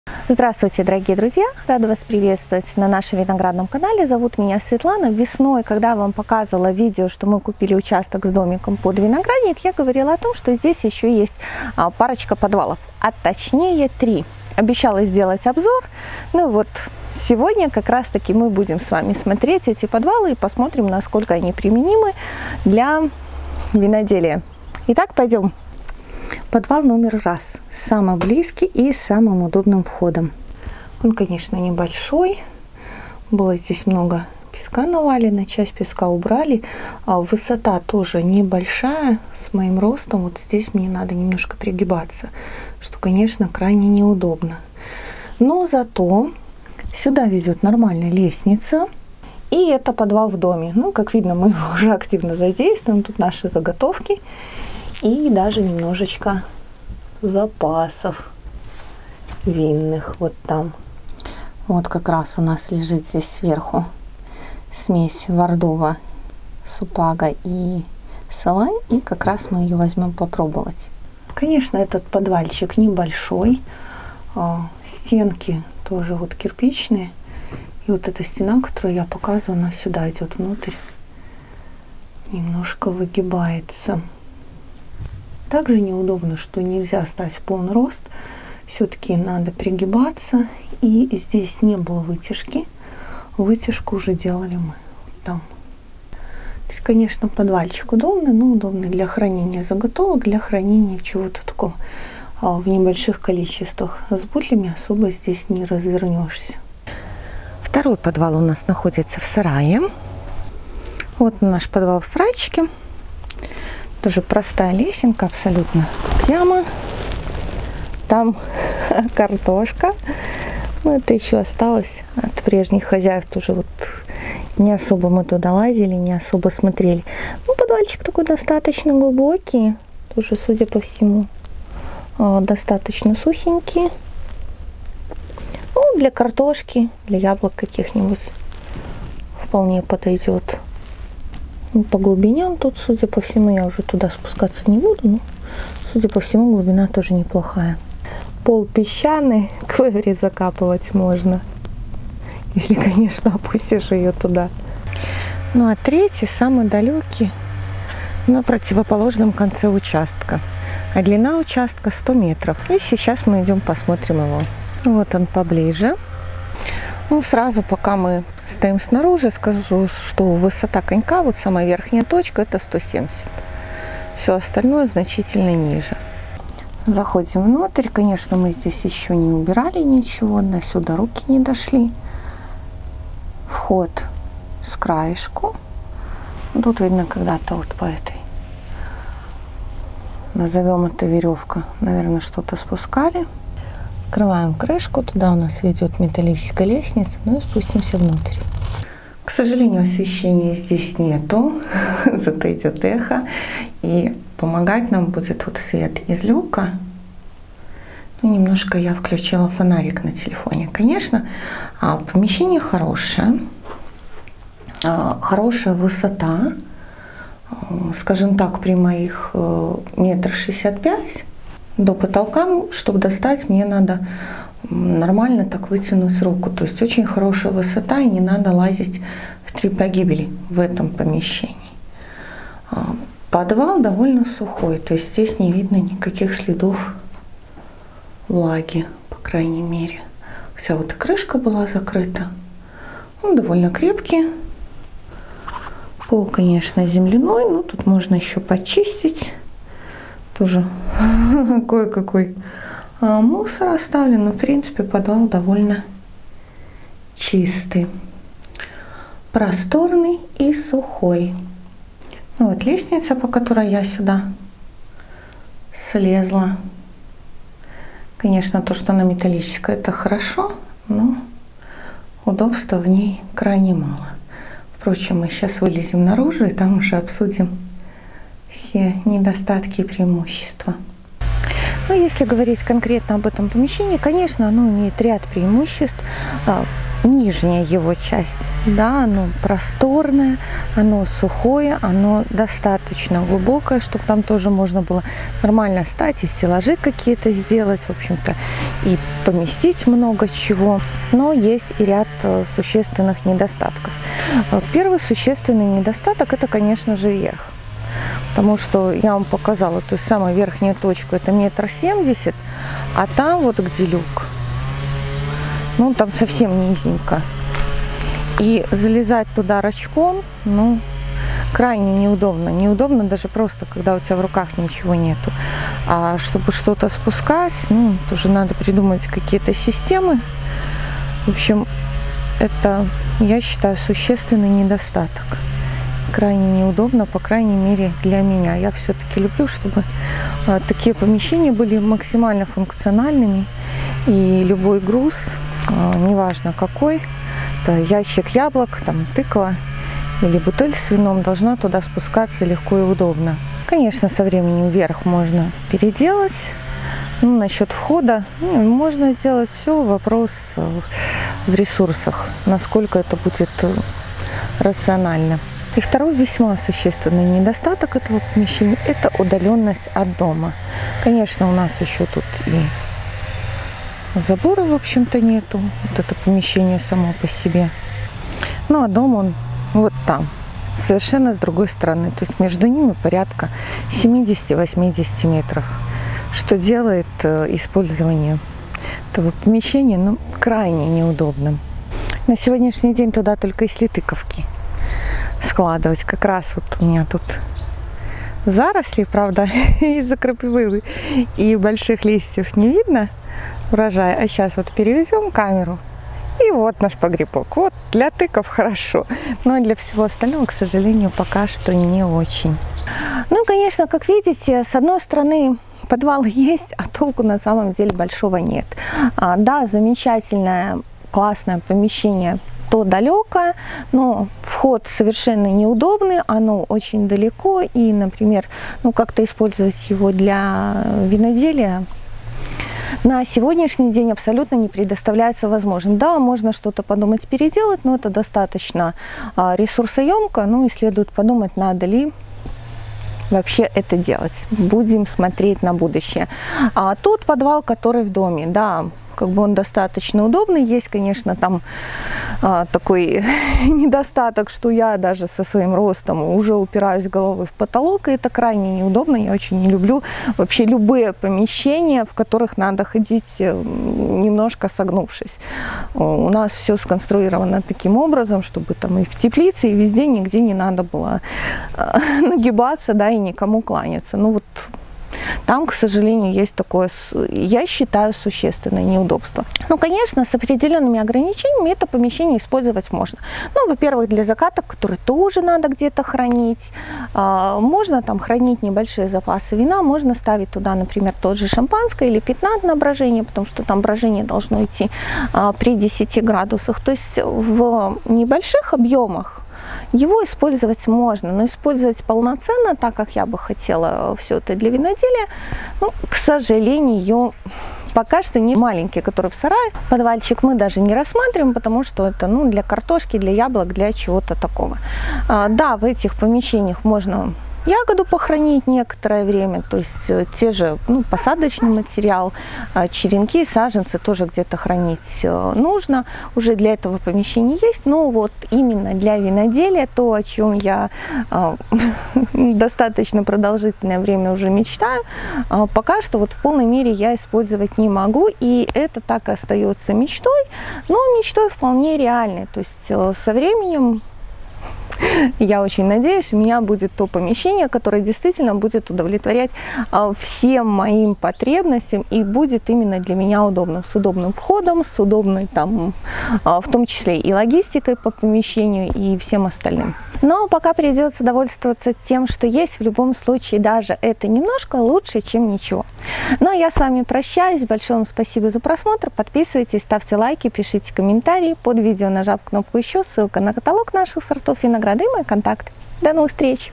Обзор наших подвальных помещений
Весной, показывая наш новый участок, я рассказывала, что там есть парочка подвальных помещений.
Посмотрим, что они из себя представляют и насколько они пригодны для целей виноделия